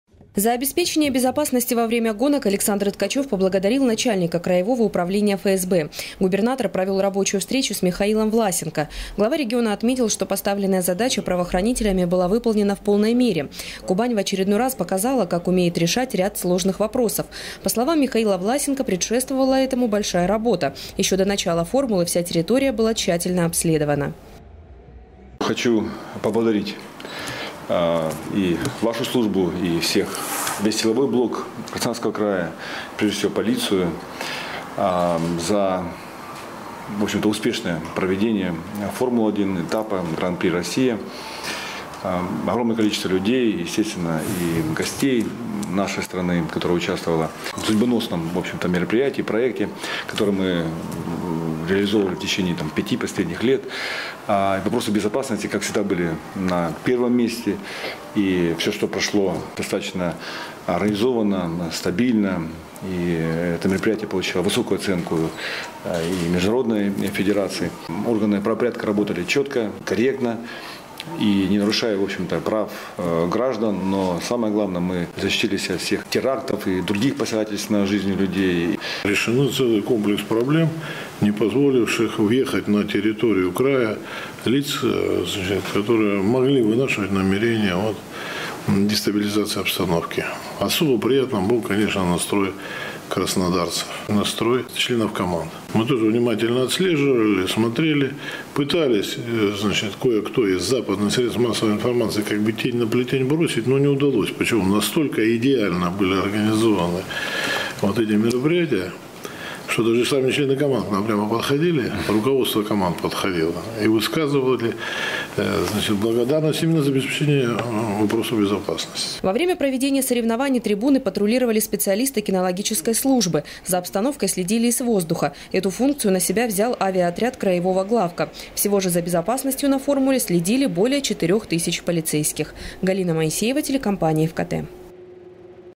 Первая в России "Формула - 1" прошла на высшем уровне. Новости 24 Сочи
Такую оценку соревнованиям дала международная комиссия.
Сочи. Новости.
Телеканал Эфкате.